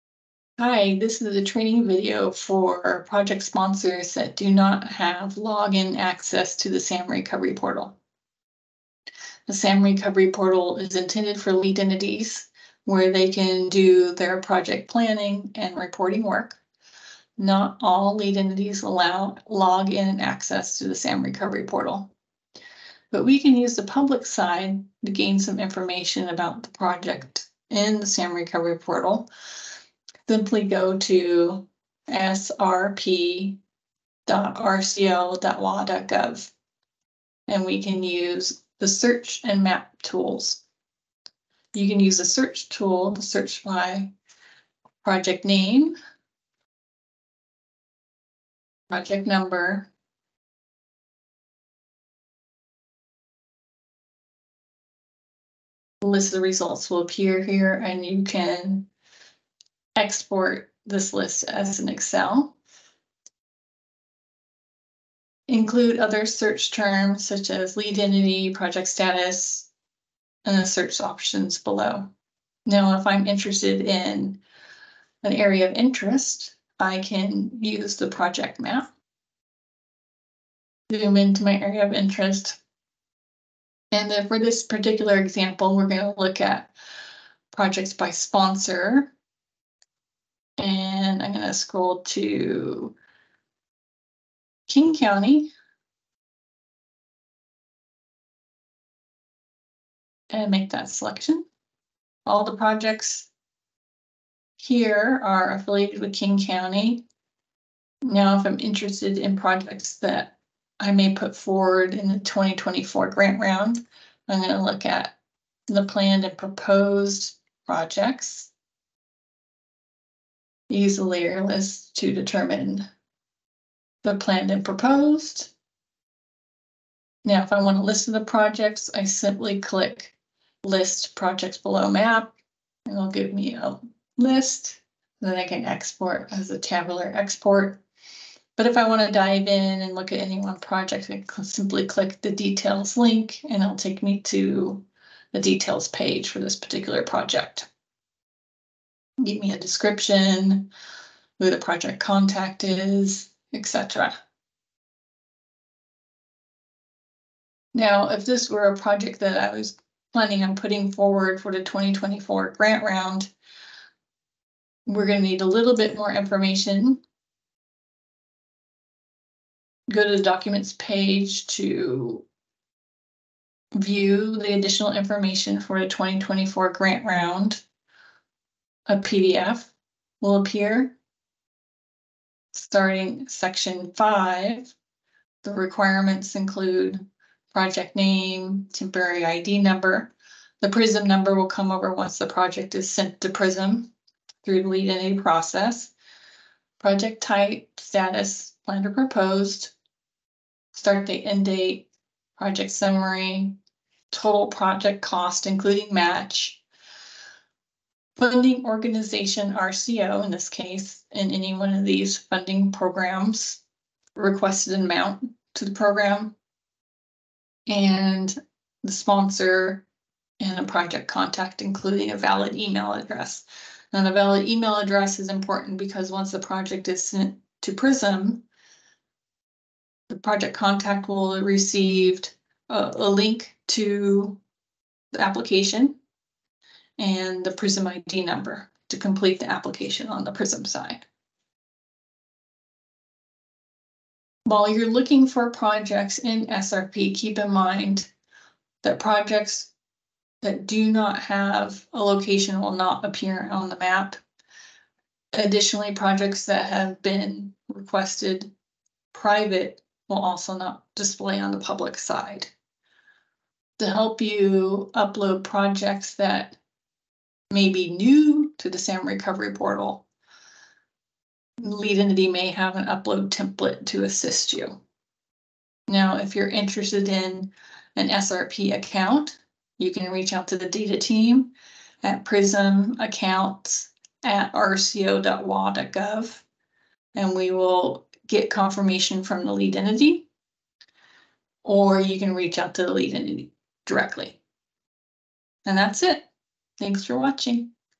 How to Navigate Salmon Recovery Portal (Public) for Sponsors
This video highlights how to find various project information for project sponsors that do not have sign-in access to Salmon Recovery Portal (SRP).